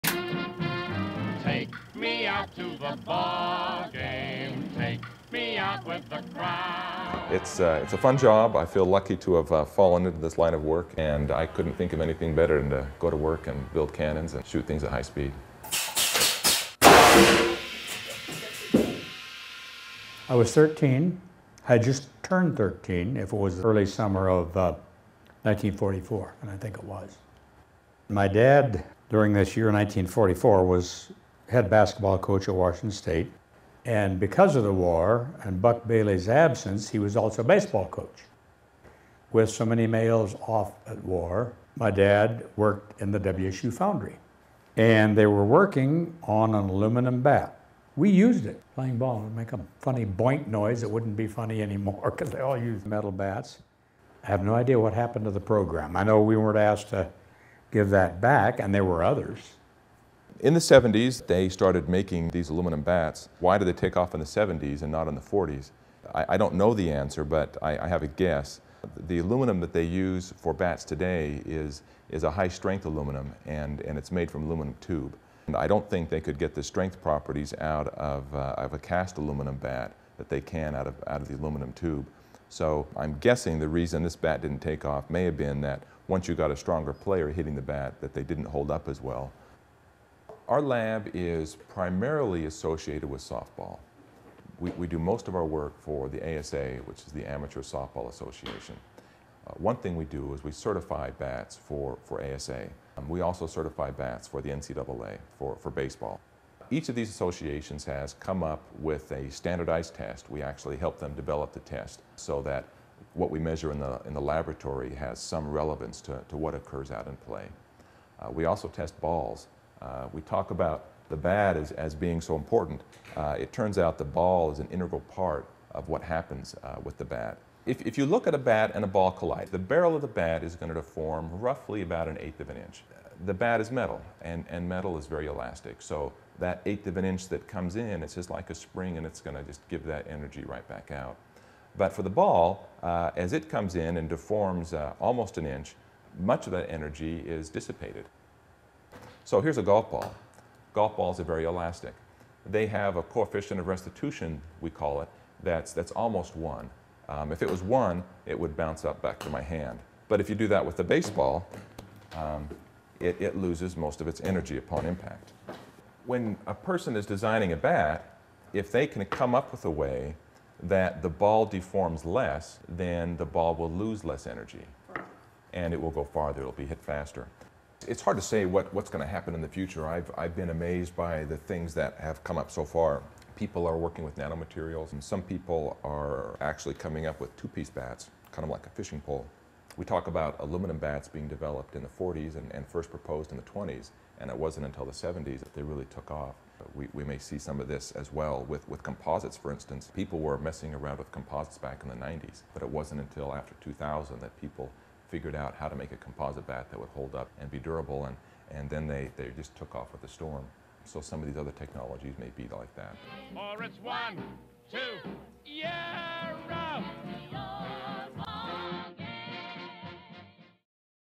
Aluminum Baseball Bats: WSU Challenge and Legacy
Aluminum baseball bats were produced in 1944 at Washington State University. Wally Friel, retired Pullman attorney and judge recalls playing baseball with them during WWII in Pullman, Wash. Researcher, Lloyd Smith, director of the WSU Sports Science Laboratory, discusses the mechanics of ball and bat performance.